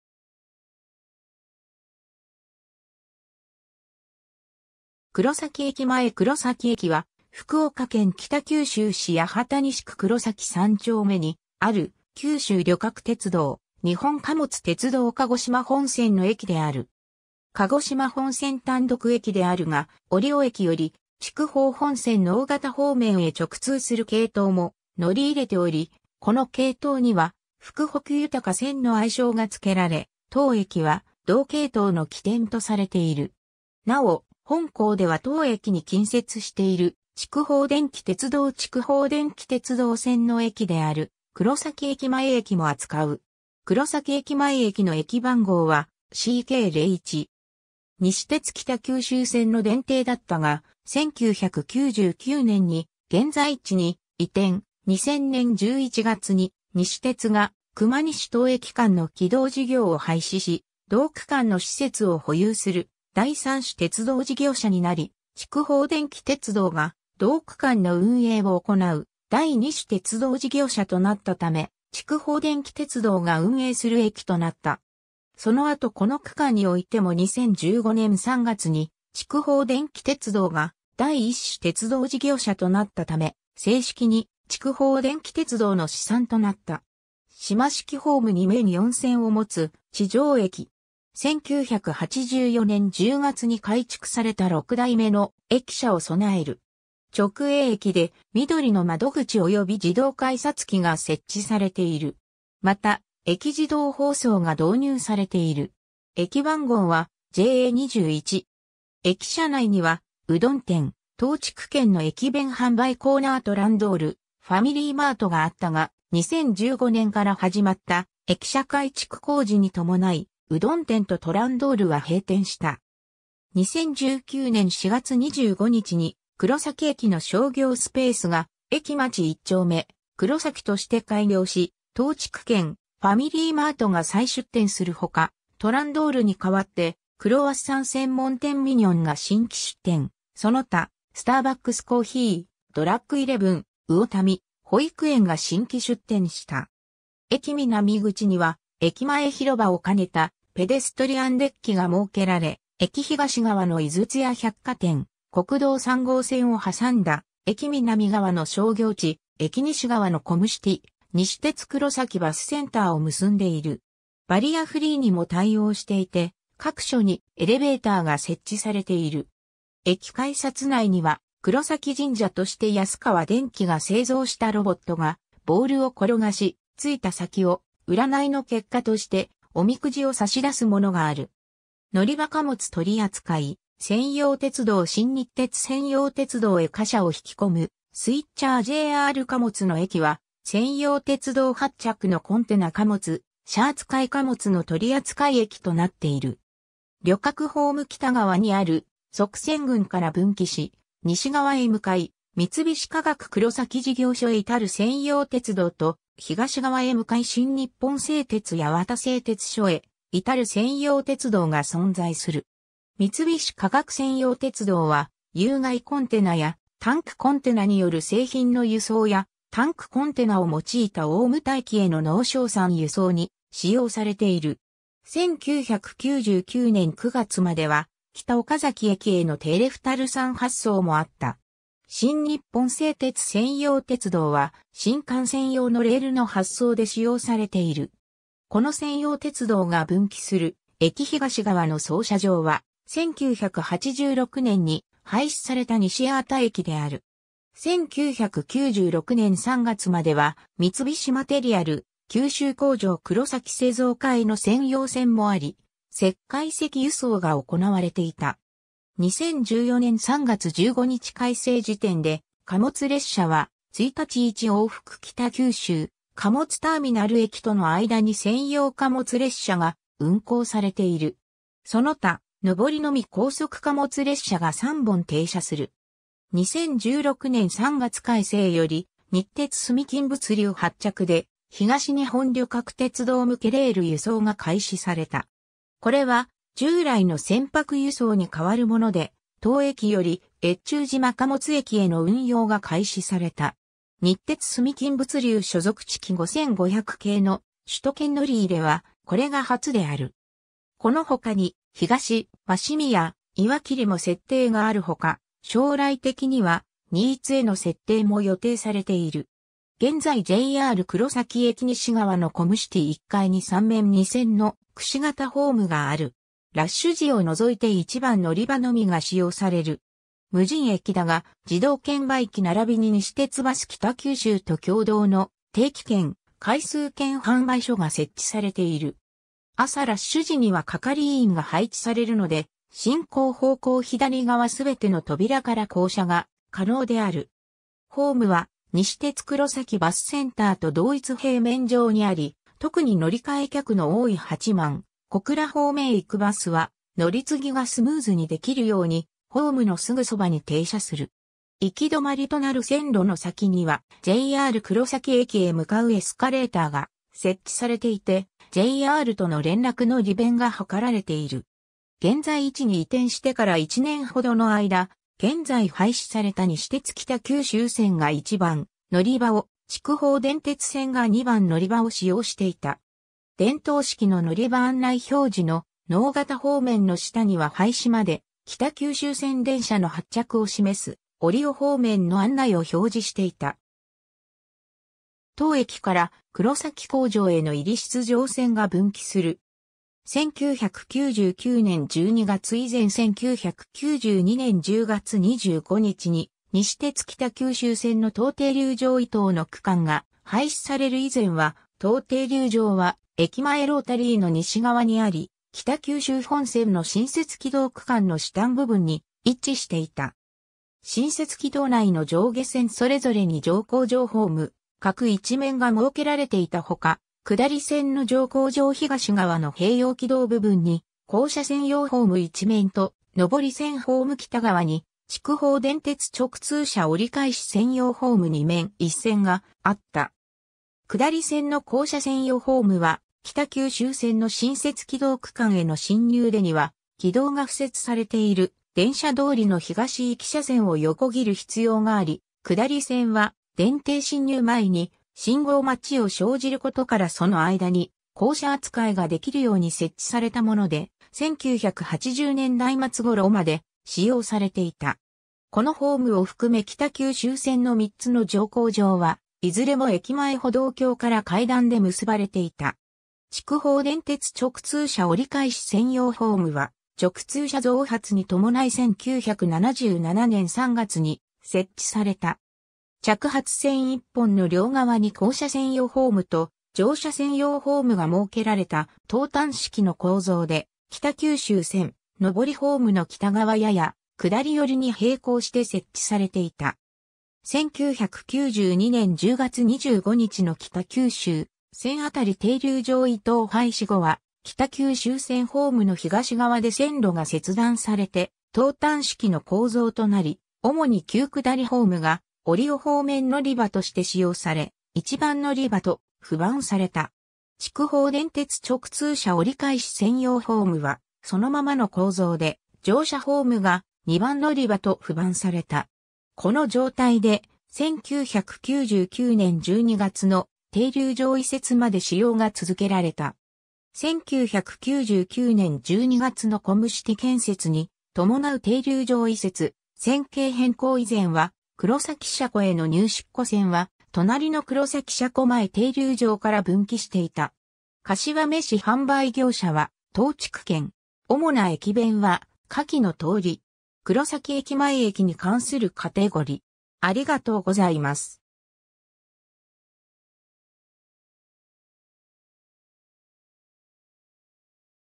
黒崎駅
黒崎駅前（2017年2月撮影） 黒崎駅（くろさきえき）は、福岡県北九州市八幡西区黒崎三丁目にある、九州旅客鉄道（JR九州）・日本貨物鉄道（JR貨物）鹿児島本線の駅である。
鹿児島本線単独駅であるが、折尾駅より筑豊本線直方方面へ直通する系統も乗り入れており、この系統には「福北ゆたか線」の愛称が付けられ、当駅は同系統の起点とされている。
 なお、本稿では当駅に近接している筑豊電気鉄道（筑豊電鉄）筑豊電気鉄道線の駅である黒崎駅前駅（くろさきえきまええき）も扱う。
黒崎駅前駅の駅番号は CK01。
 西鉄北九州線の電停だったが、1999年に現在地に移転、2000年11月に西鉄が熊西 - 当駅間の軌道事業を廃止し同区間の施設を保有する第三種鉄道事業者になり、筑豊電気鉄道が同区間の運営を行う第二種鉄道事業者となったため筑豊電気鉄道が運営する駅となった。
その後この区間においても2015年3月に筑豊電気鉄道が第一種鉄道事業者となったため、正式に筑豊電気鉄道の資産となった。
 島式ホーム2面4線を持つ地上駅。
1984年（昭和59年）10月に改築された6代目の駅舎（橋上駅舎）を備える。
直営駅でみどりの窓口および自動改札機が設置されている。
また、駅自動放送が導入されている。
駅番号はJA21。
 駅舎内にはうどん店・東筑軒の駅弁販売コーナー・トランドール、ファミリーマートがあったが、2015年から始まった駅舎改築工事に伴い、うどん店とトランドールは閉店した。
 2019年4月25日に黒崎駅の商業スペースが「えきマチ1丁目黒崎」として開業し、東筑軒・ファミリーマート（銘品蔵併設）が再出店するほか、トランドールに代わって、クロワッサン専門店ミニョンが新規出店、その他、スターバックスコーヒー、ドラッグイレブン、魚民、保育園が新規出店した。
 駅南口には駅前広場を兼ねたペデストリアンデッキが設けられ、駅東側の井筒屋百貨店、国道3号線を挟んだ駅南側の商業地、駅西側のコムシティ、西鉄黒崎バスセンターを結んでいる。
バリアフリーにも対応していて、各所にエレベーターが設置されている。
 駅改札内には「黒崎神社」として安川電機が製造したロボットがボールを転がし、着いた先を占いの結果としておみくじを差し出すものがある。
 のりば 貨物取扱・専用鉄道 新日鐵専用鉄道へ貨車を引き込むスイッチャー（2009年1月） JR貨物の駅は、専用鉄道発着のコンテナ貨物・車扱貨物の取扱駅となっている。
 旅客ホーム北側にある側線群から分岐し、西側へ向かい三菱化学黒崎事業所へ至る専用鉄道と、東側へ向かい新日本製鐵八幡製鐵所へ至る専用鉄道が存在する。
三菱化学専用鉄道は、有蓋コンテナやタンクコンテナによる製品の輸送や、タンクコンテナを用いた大牟田駅への濃硝酸輸送に使用されている。
1999年9月までは、北岡崎駅へのテレフタル酸発送もあった。
新日本製鐵専用鉄道は新幹線用のレールの発送で使用されている。
この専用鉄道が分岐する駅東側の操車場は、1986年に廃止された西八幡駅である。
 1996年3月までは三菱マテリアル九州工場黒崎製造課への専用線もあり、石灰石輸送が行われていた。
 2014年3月15日改正時点で貨物列車は、1日1往復北九州貨物ターミナル駅との間に専用貨物列車が運行されている。
その他、上り（北九州貨物ターミナル駅行き）のみ高速貨物列車が3本（うち、当駅始発・終着が1本ずつ）停車する。
 2016年3月改正より、日鉄住金物流発着で東日本旅客鉄道向けレール輸送が開始された。
これは従来の船舶輸送に変わるもので、当駅より越中島貨物駅（東京レールセンター）への運用が開始された。
日鉄住金物流所属チキ5500形の首都圏乗り入れは、これが初である。
このほかに東鷲宮、岩切（仙台レールセンター）も設定があるほか、将来的には新津への設定も予定されている。
 現在（1999年12月以降） JR黒崎駅西側のコムシティ1階に3面2線の櫛形ホームがある。
ラッシュ時を除いて1番のりばのみが使用される。
無人駅だが、自動券売機ならびに西鉄バス北九州と共同の定期券・回数券販売所が設置されている。
朝ラッシュ時には係員が配置されるので進行方向左側すべての扉から降車が可能である。
 ホームは西鉄黒崎バスセンターと同一平面上に有り、特に乗り換え客の多い八幡・小倉方面...